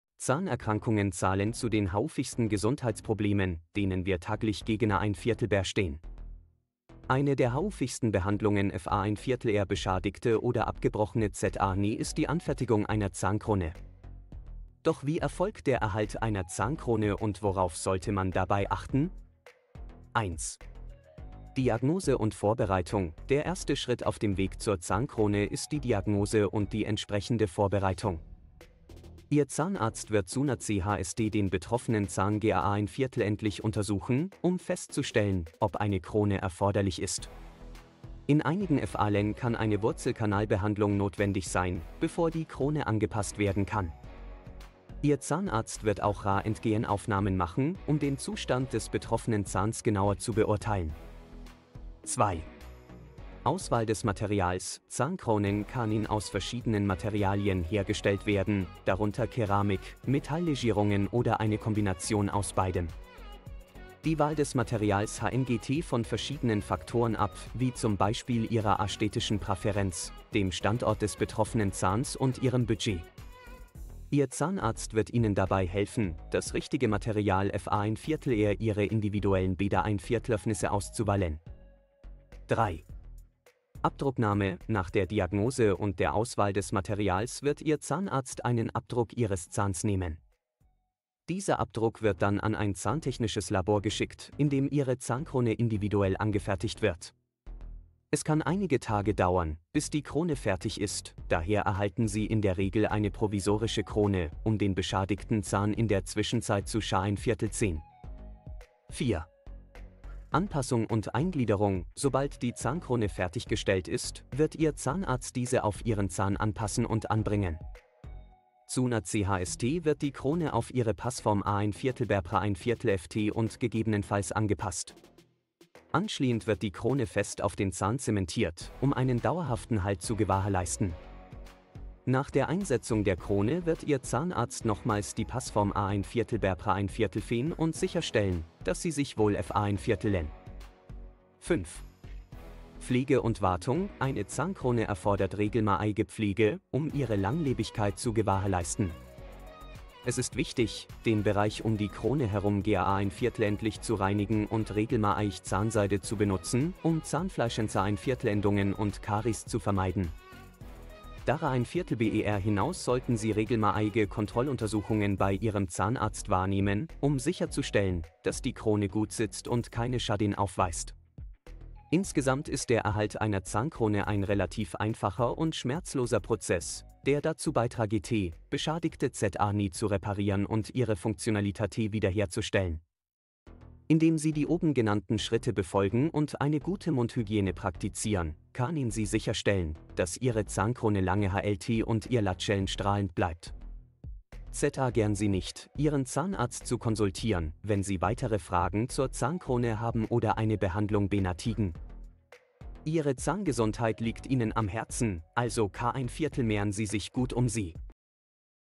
Die perfekte Lösung für beschädigte Zähne - Wie funktioniert der Erhalt einer Zahnkrone? | Zahn
Zahnerkrankungen zählen zu den häufigsten Gesundheitsproblemen, denen wir täglich gegenüberstehen. Eine der häufigsten Behandlungen für beschädigte oder abgebrochene Zähne ist die Anfertigung einer Zahnkrone. Doch wie erfolgt der Erhalt einer Zahnkrone und worauf sollte man dabei achten? Zahnkrankheiten, Gesundheitsprobleme, Behandlung, beschädigte Zähne, abgebrochene Zähne, Zahnkr